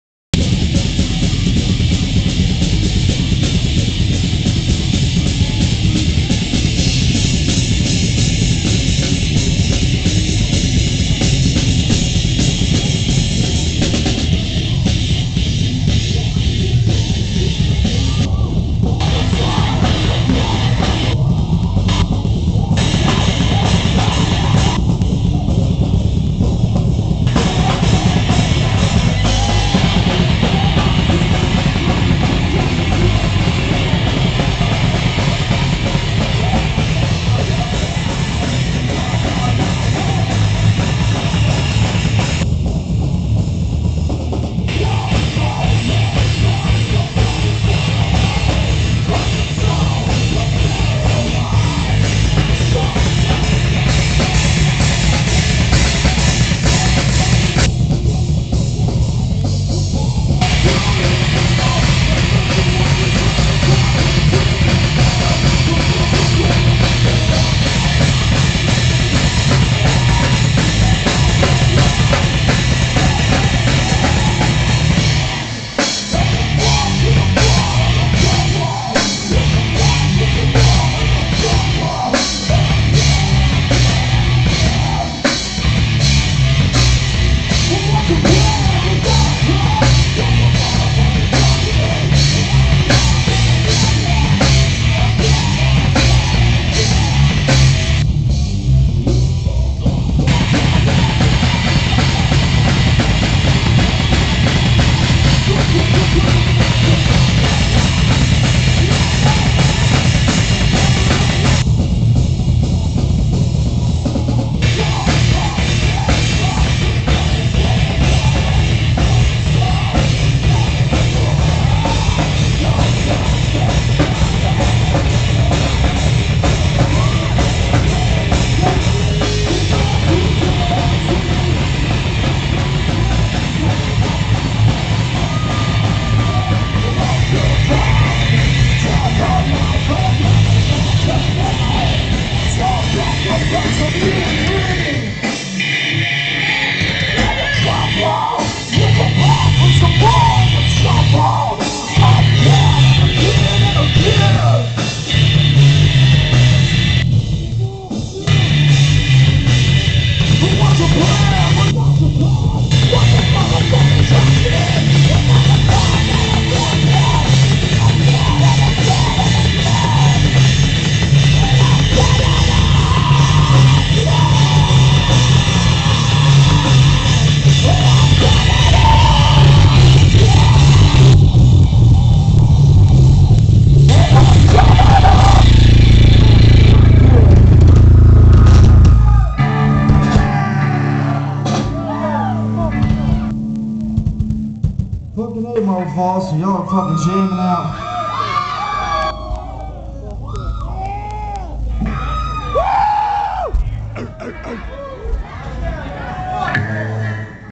MACAVA Live in Marble Falls 2007
Trent Gumbert, Corey and Brett Bryant, Chad Upton killing it at the Marble Falls Metal Fest